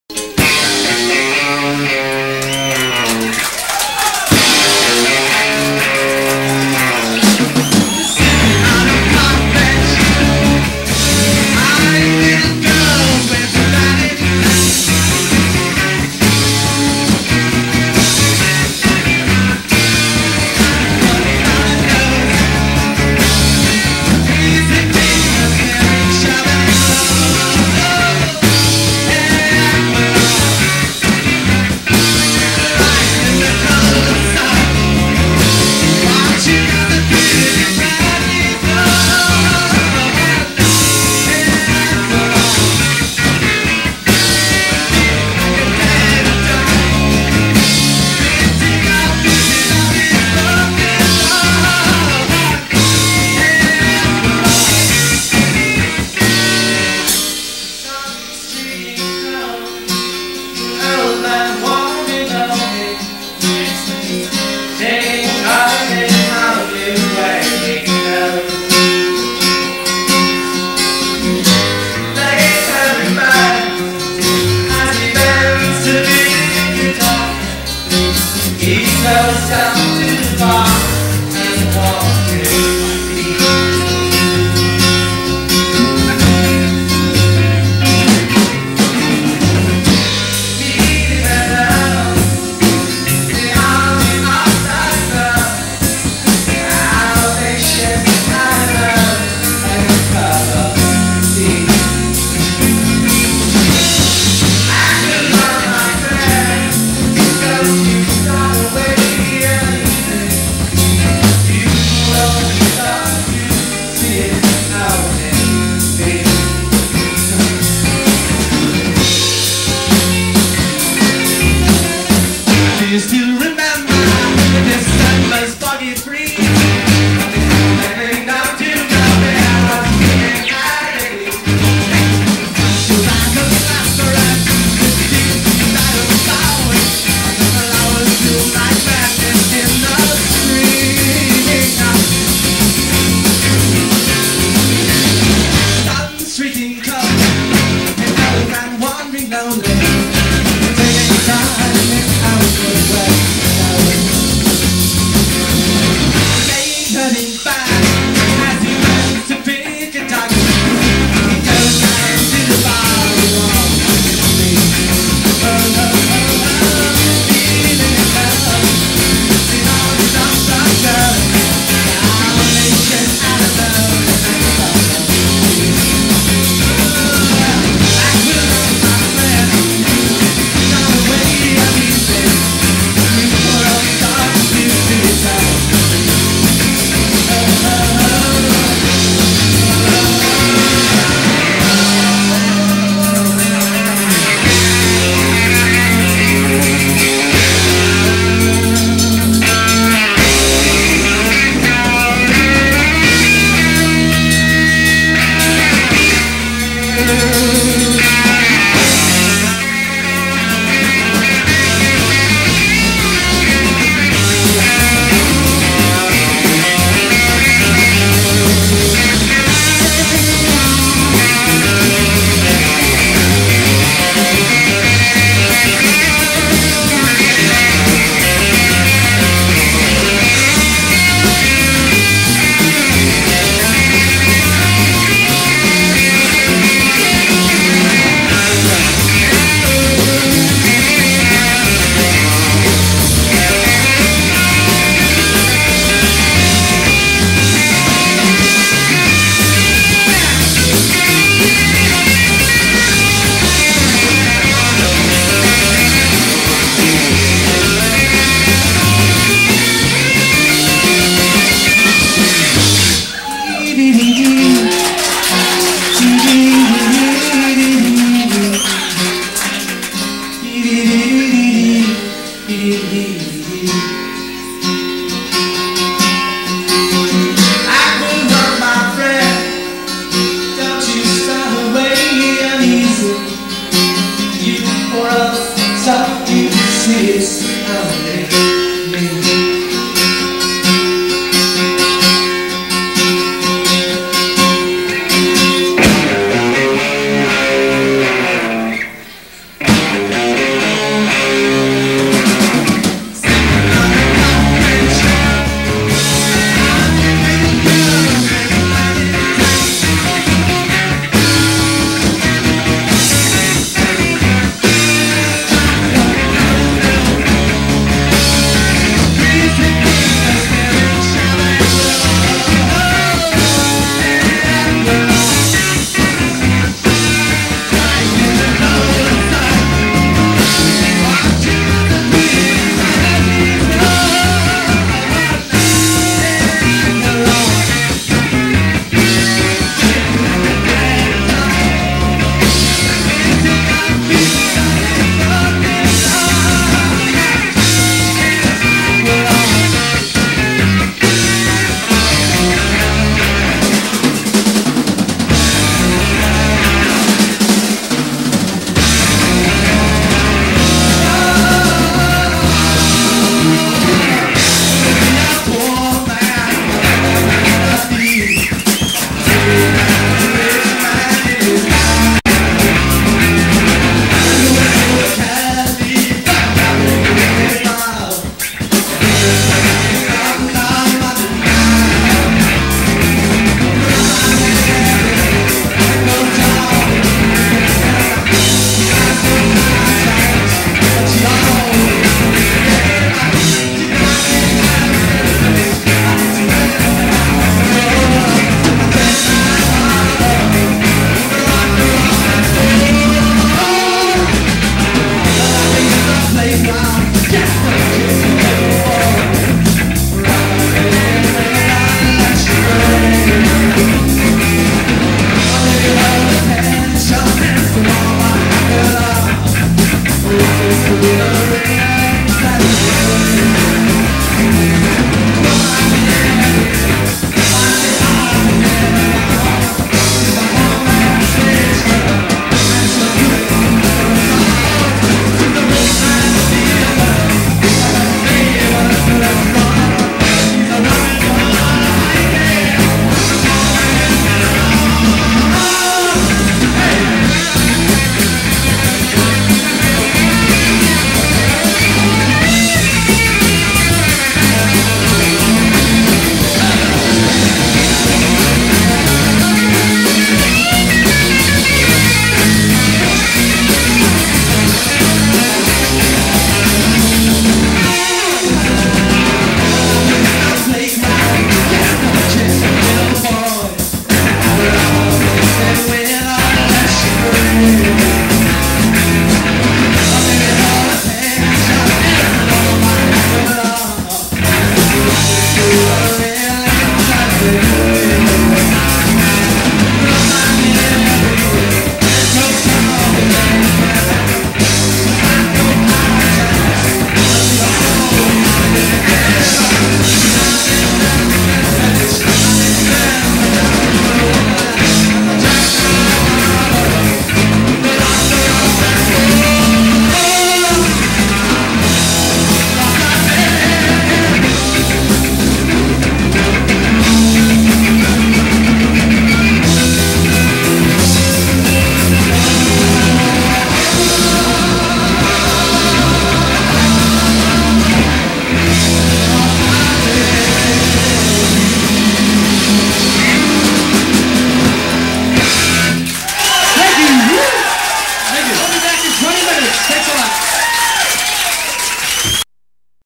Earthwood - Tull Medley pt 02
Earthwood plays their Jethro Tull medley to an eager crowd @ Chicago's Rusty Nail circa 1977.  Sorry, but since it is over 10 minutes I had no choice but to break it into two parts. The Tube rejected it in its entirety as a single video clip.